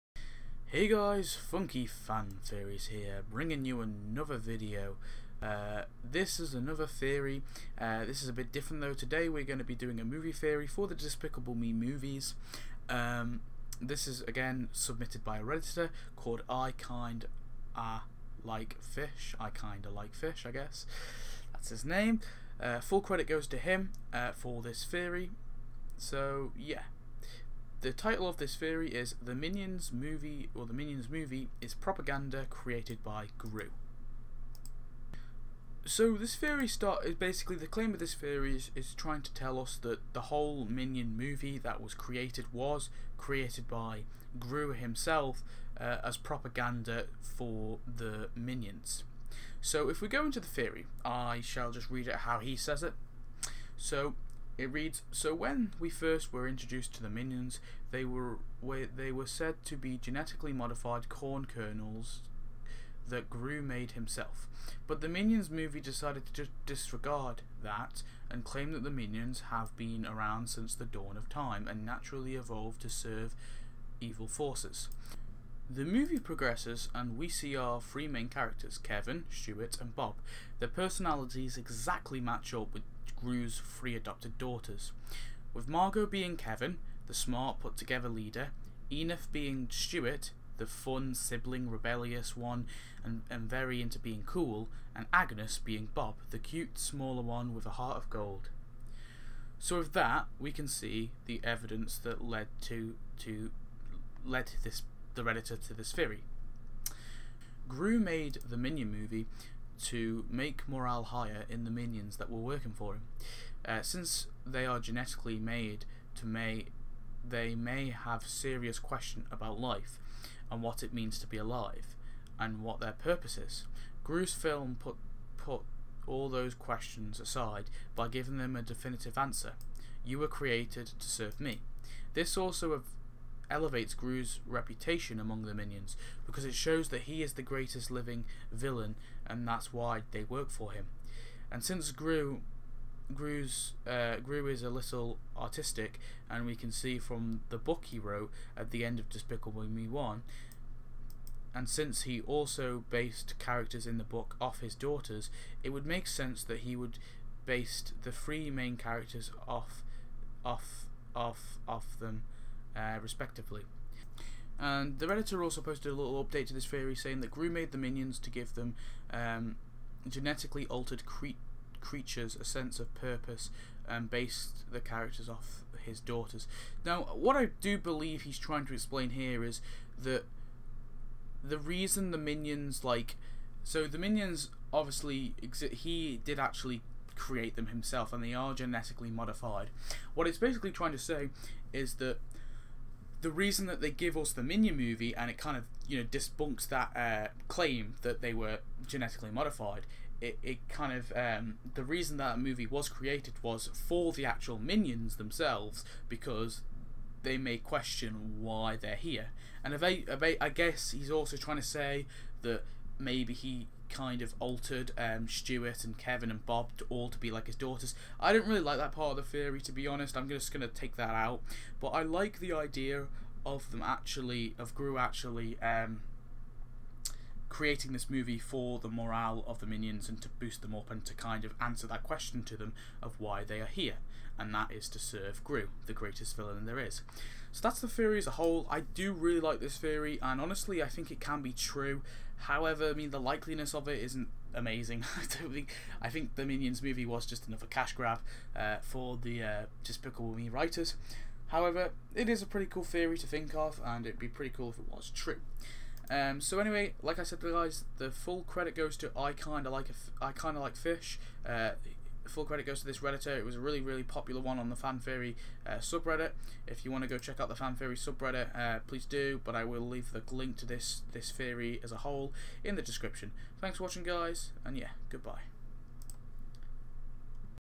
Movie Fan Theory | The Minions Movie is Propaganda Created By Gru
Read the full theory

So when we first were introduced to the Minions, they were said to be genetically modified corn kernels that Gru made himself, but The Minions movie decided to disregard that and claim that minions have been around since the dawn of time and naturally evolved to serve evil forces. The movie progresses and we meet our three main characters: Kevin, Stuart, and Bob. Their personalities exactly match up with Gru's three adopted daughters with Margo being Kevin, the smart put together leader, Edith being Stuart, the fun slightly rebellious one and very into being cool, and Agnes being Bob, the cute smaller one with a heart of gold. So with that we can see the evidence that led me to this theory.
Gru made the Minion movie to make morale higher in the minions that are working for him. Since they are genetically made they may have serious questions about life and what it means to be alive and what their purpose is. Gru's film puts all those questions aside by giving them a definitive answer: you were created to serve me. This also elevates Gru's reputation among the minions because it shows that he is the greatest living villain and that's why the work for him. And since Gru is a little artistic as we can see from the book he wrote at the end of Despicable Me 1, and since he also based characters in that book off of his daughters, it would make sense that he based the three main characters off of them respectively.
TL;DR: Gru made The Minions to give genetically altered creatures a sense of purpose and based the characters off of his daughters.